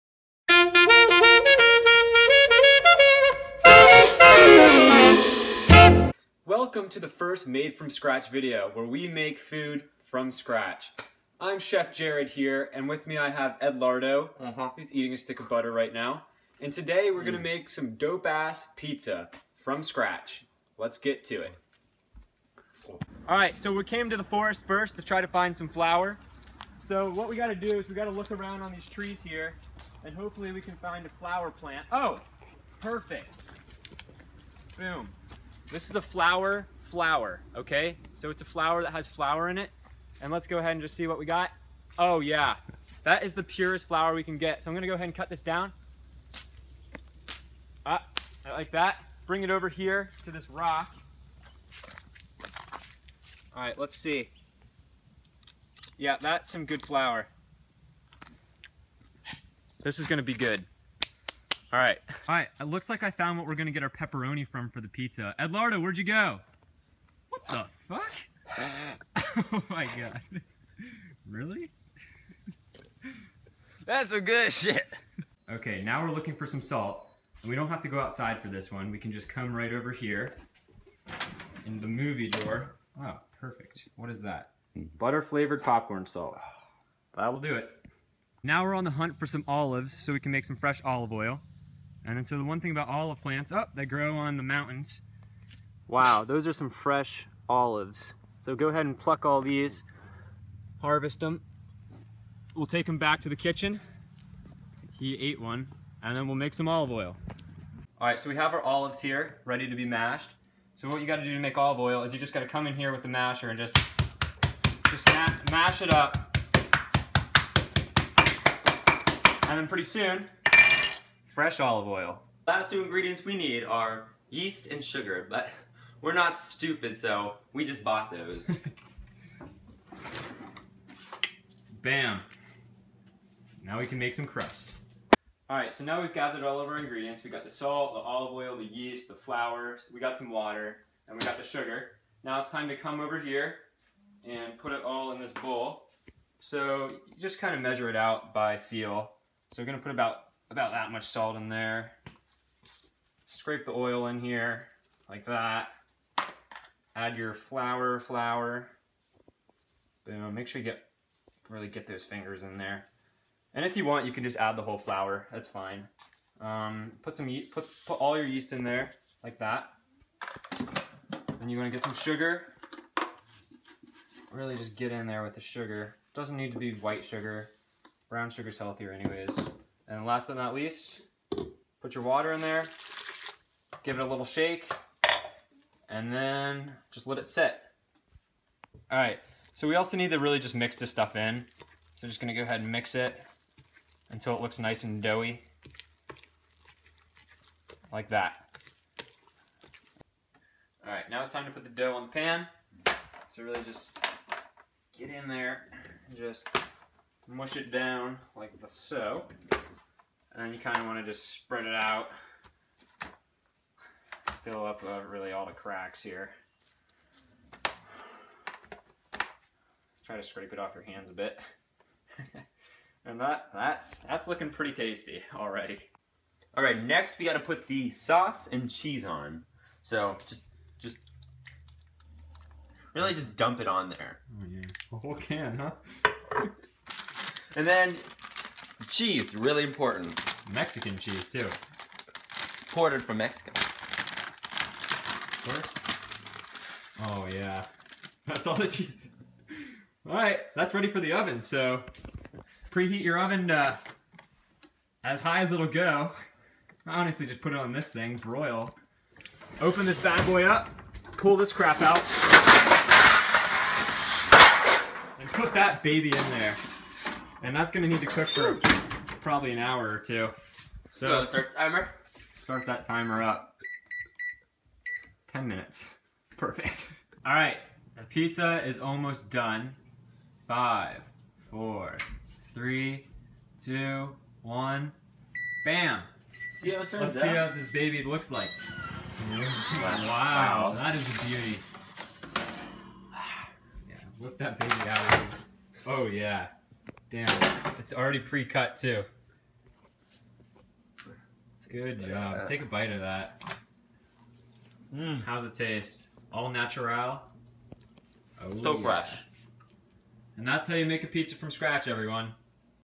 Made From Scratch: Easy Peasy Pizza
We show you how to scavenge the wild for ingredients to make your own pizza from scratch!